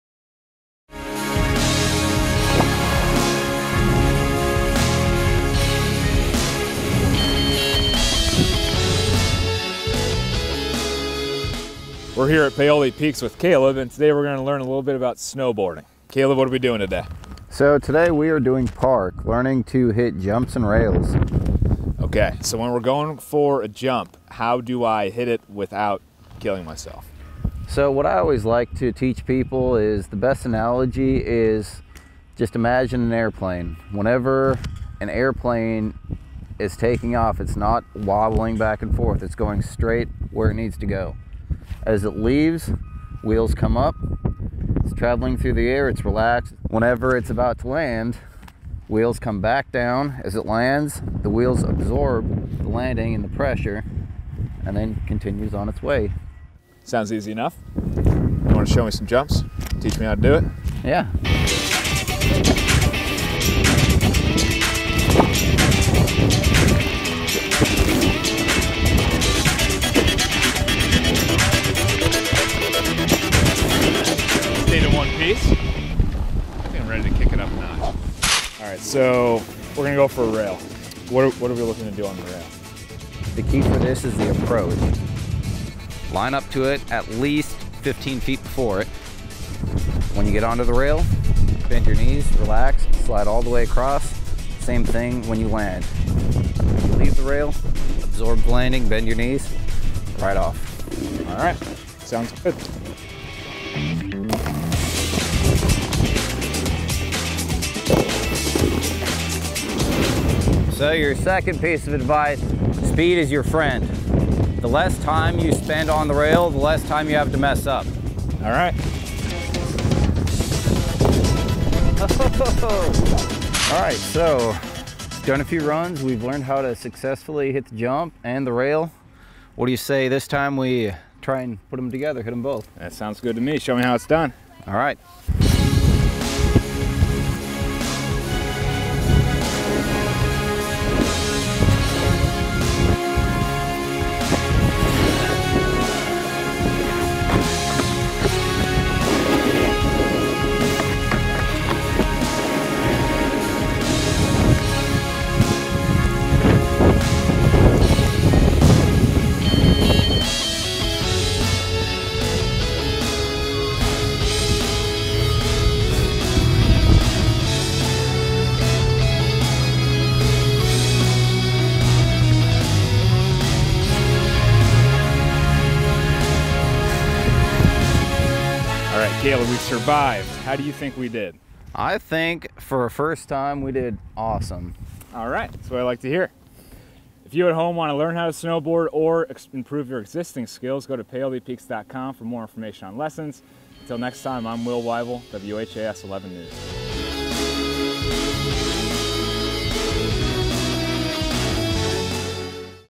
Weible's World of Winter Olympic Sports: Snowboard Slopestyle
Ahead of the 2018 PyeongChang Winter Olympics, WHAS11 Digital Reporter Will Weible took to the slopes and hit the ice to find out what it takes to compete in the Winter Olympic Games.